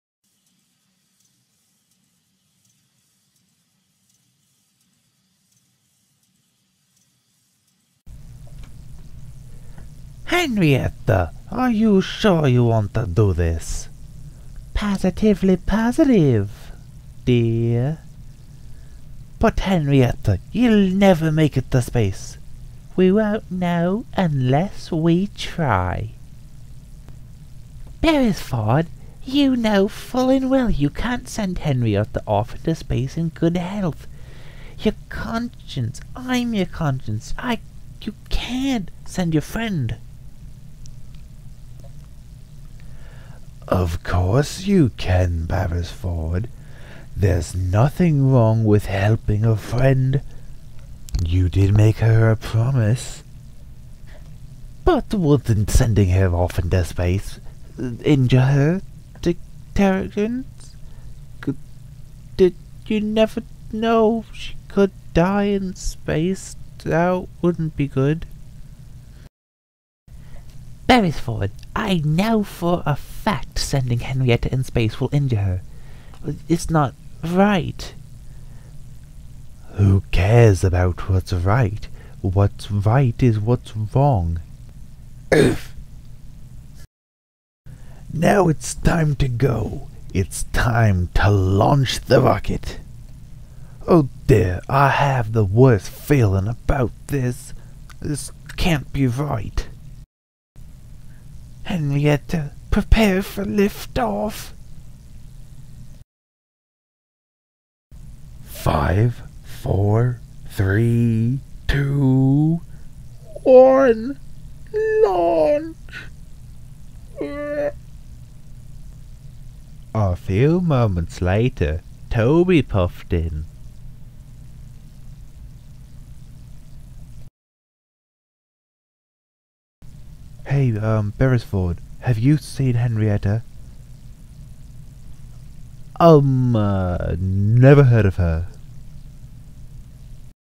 Beresford and the Launch
Something I made spontaneously the other day when I was messing around with some of my trains. I lost interest in making this "thing" pretty quick hence why the quality starts to dip towards the end of the video. (No sound effects, etc.). Anyway, enjoy.